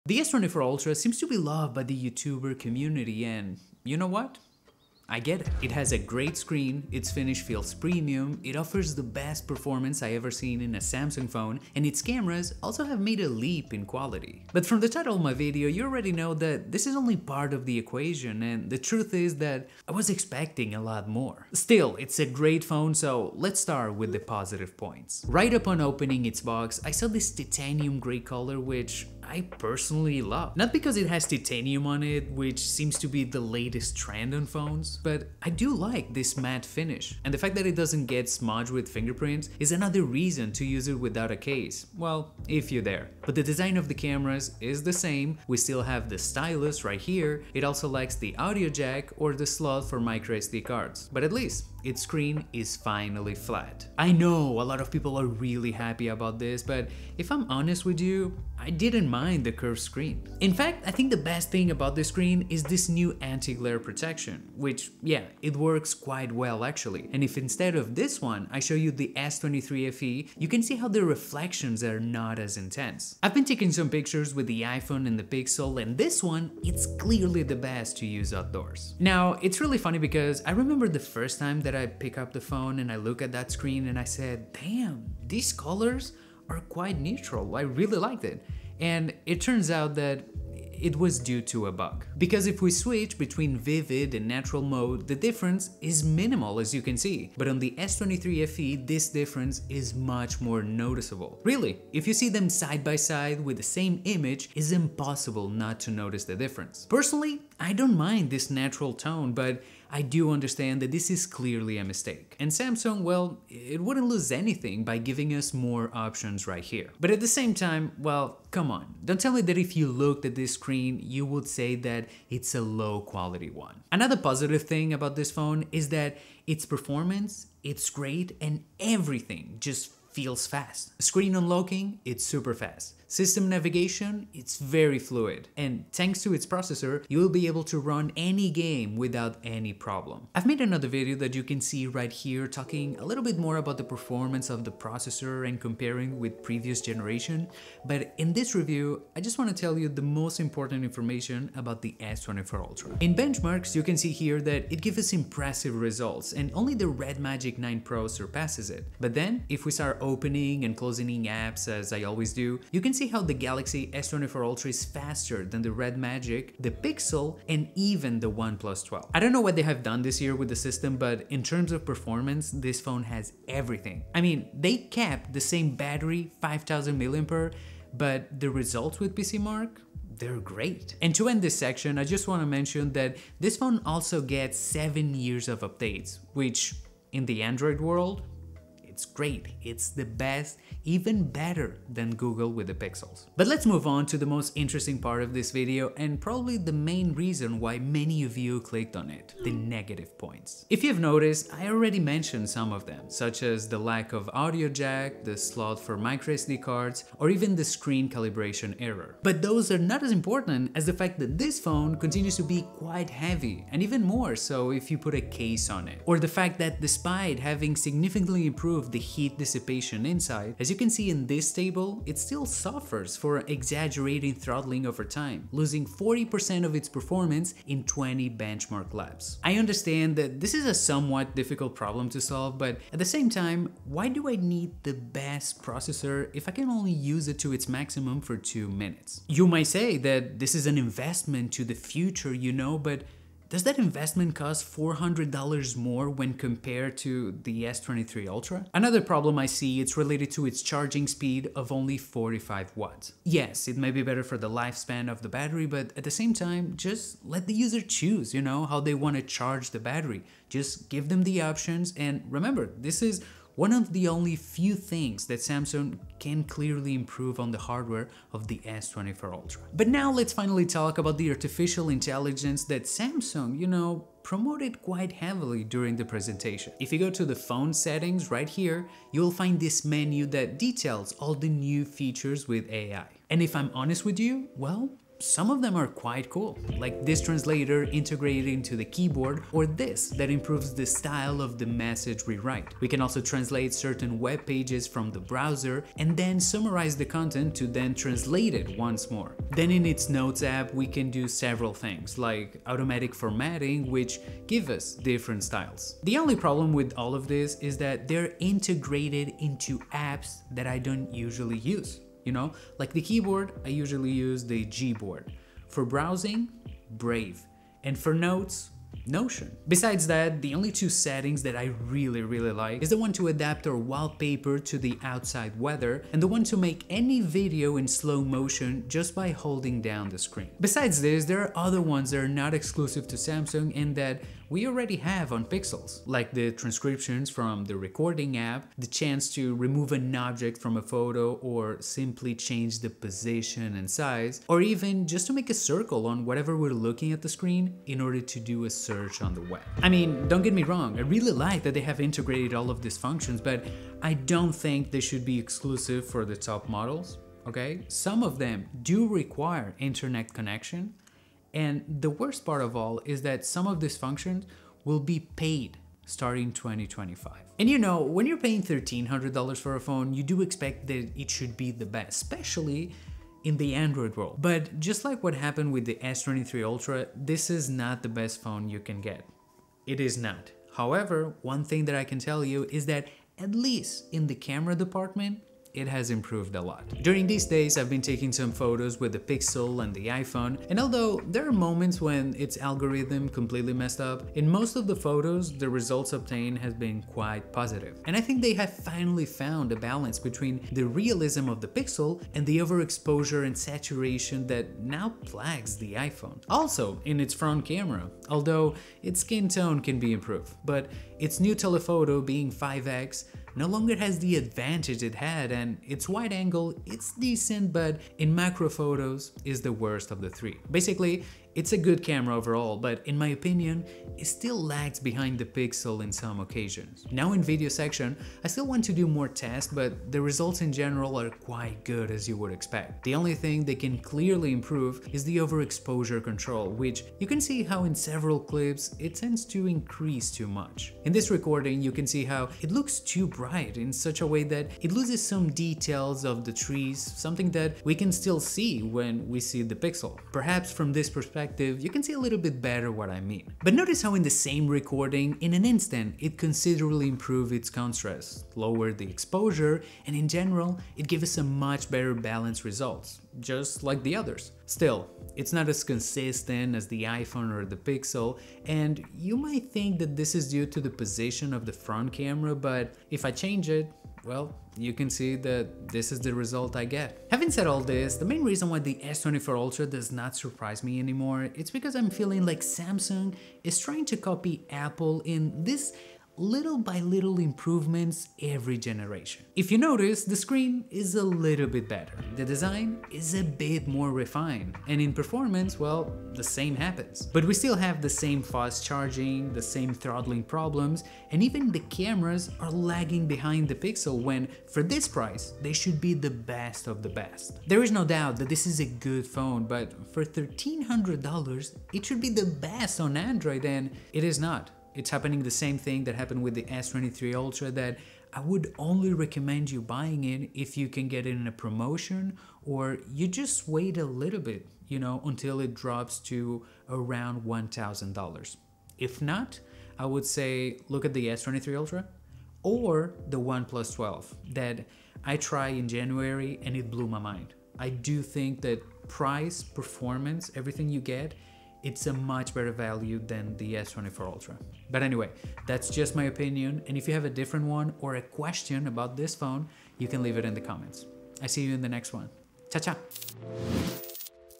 WHAT HAPPENED to Samsung? | Galaxy S24 Ultra Review
The Samsung Galaxy S24 Ultra is a phone that brings almost nothing new. A new screen that prevents reflections; new AI functions; better optimized cameras; and a better processor... But the weak points we had with the S23 Ultra are still here.

This is a GREAT phone, with a HUGE price. And it seems that Samsung wants us to buy the S23 Ultra, as the price difference becomes increasingly difficult to justify. Samsung knows what it needs to improve, but follows Apple's lead and gives us small periodic improvements.

📱Spigen Cases:

00:00 - Intro00:24 - The Positive03:14 - The Negative04:38 - New AI Features06:17 - Fotos & Videos08:54 - Conclusion